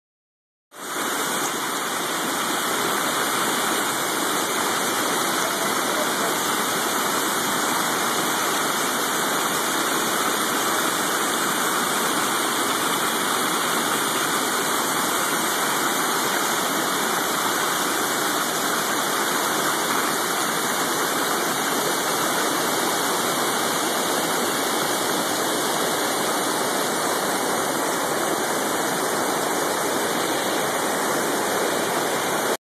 Billo Falls, IL #1
It rained a lot in south suburban Chicago on 6-9-2011. This creek is about 8 feet below the driveway most of the rear, today it was more than a foot over the top of the bridge.